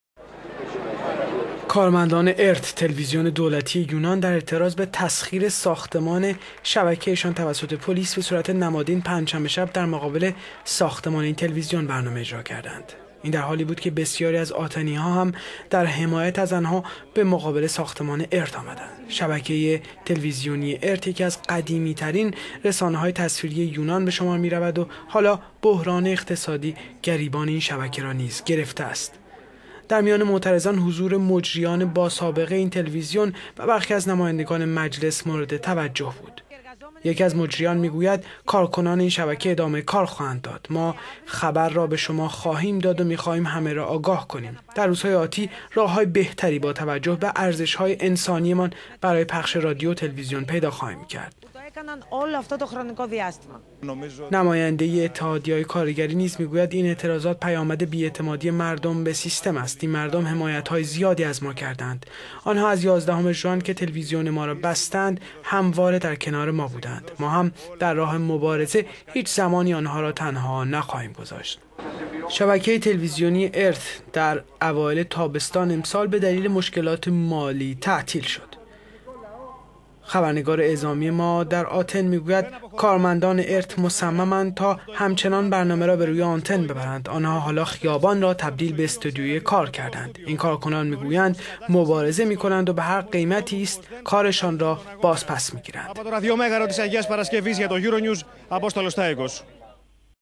کارکنان اخراجی تلویزیون «ارتِ» یونان خیابان را تبدیل به استودیوی کار خود کردند
کارمندان ارت، تلویزیون دولتی یونان در اعتراض به...

کارمندان ارت، تلویزیون دولتی یونان در اعتراض به تعطیلی شبکه خود و تسخیر ساختمان آن توسط پلیس یونان،  بصورت نمادین پنج شنبه شب در مقابل ساختمان ارت برنامه اجرا کردند.

این در حالی بود که بسیاری از آتنی ها هم در حمایت از آنها به مقابل ساختمان ارت آمدند. شبکه تلویزیونی ارت یکی از قدیمی ترین رسانه های تصویری یونان بشمر می رفت و حالا بحران اقتصادی گریبان این شبکه را نیز گرفته است. در میان معترضان حضور مجریان با سابقه تلویزیون دولتی ارت و برخی از نمایندگان مجلس مورد توجه بود.

یکی از مجریان این شبکه می گوید: "کارکنان این شبکه ادامه کار خواهند داد. ما خبر را به شما می دهیم و می خواهیم همه را آگاه کنیم. در روزهای آتی راههای بهتری در خور ارزشهای انسانیمان برای پخش رادیو و تلویزیون پیدا خواهیم کرد."

نماینده اتحادیه های کارگری نیز می گوید: "این اعتراضات پیامد بی اعتمادی مردم به نظام است. این مردم حمایت های زیادی از ما کرده اند. آنها از یازدهم ژوین که تلویزیون ما را بستند همواره در کنار ما بوده اند. ما هم در راه مبارزه هیچ زمانی آنها را تنها نخواهیم گذاشت."

کارمندان ارت می گویند در خیابان می مانند و بروی آنتن می روند. آنها حالا خیابان را تبدیل به استودیوی کار کرده اند. این کارکنان می گویند مبارزه می کنند و به هرقیمتی کار شان را باز پس می گیرند.